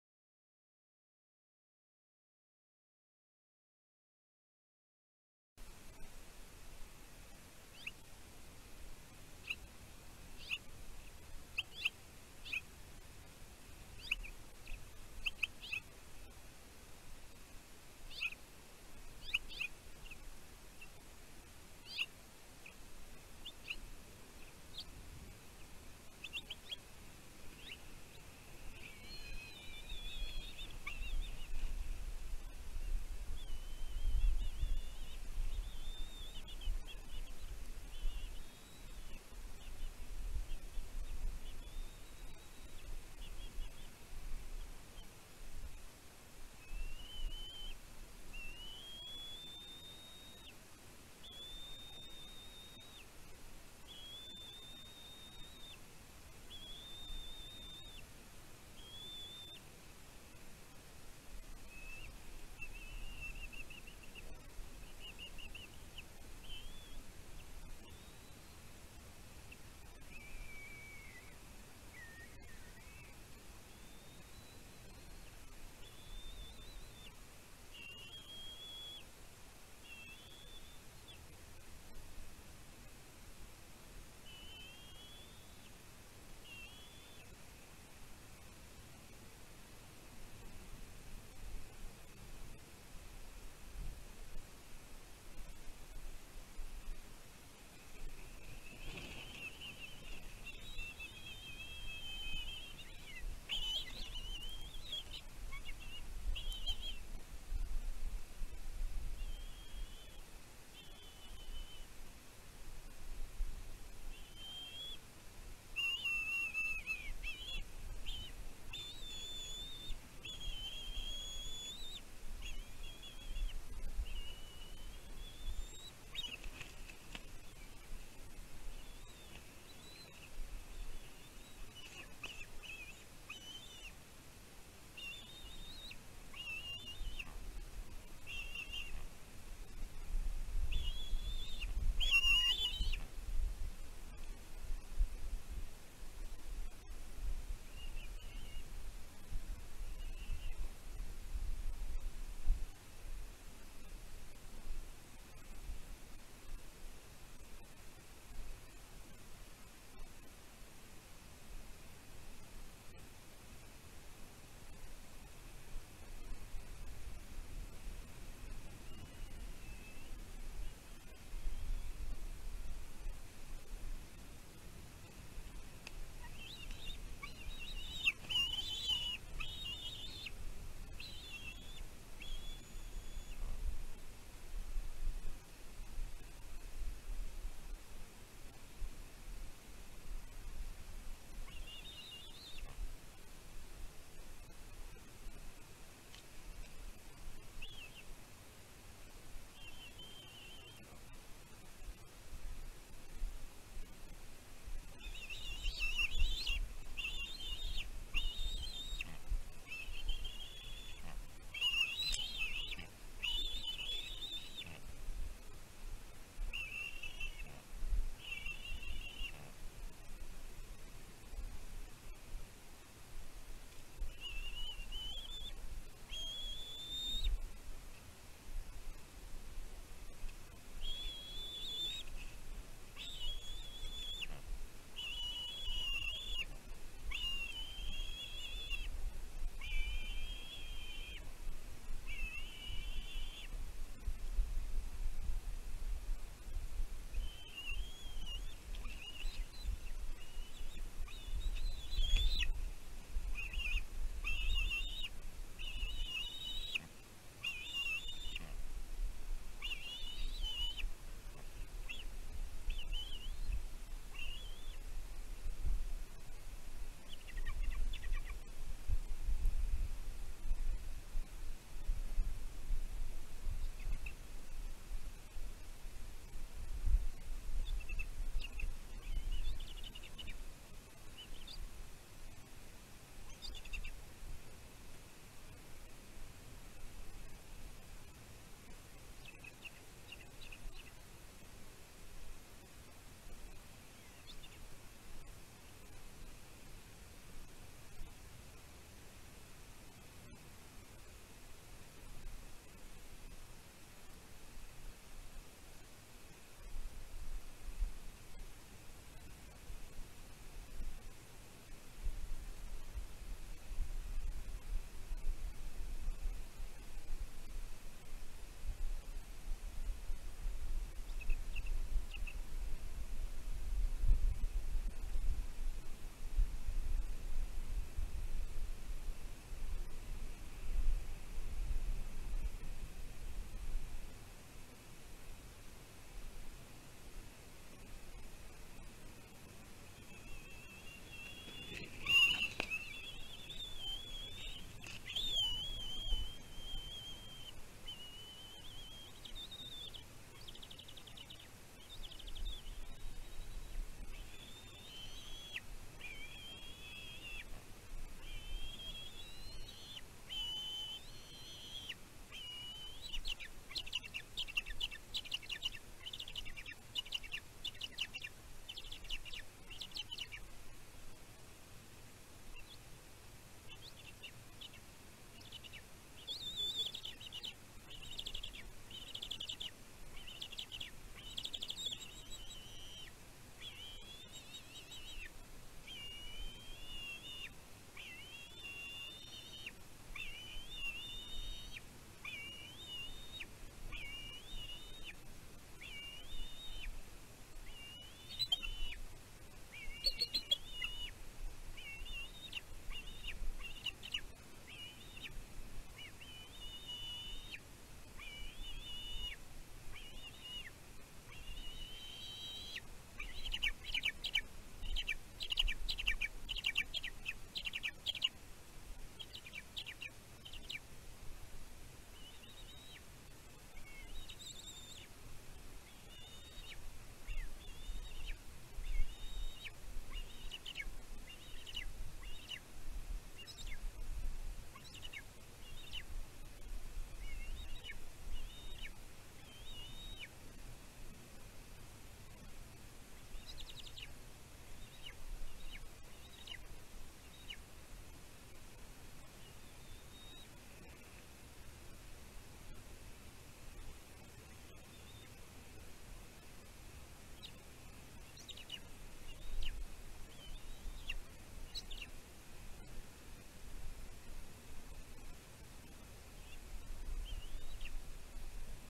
Two Harbors Cam I. Anthony have a lot to say about Mom,s deliveries. 06.15. 2023. Explore.org
The nest cam is powered by Explore.org.
The Two Harbors nest cam is on Catalina Island, CA, USA
This bald eagle pair, K-81, Chase, and K-82, Cholyn have built this new nest for some years ago. They used the old Two Harbors nest for many years. The West End female eagle, K-91, Thunder, hatched on the Two Harbors nest in 2009.
Cholyn and Chase have one offspring in 2023, 49/D, named Anthony.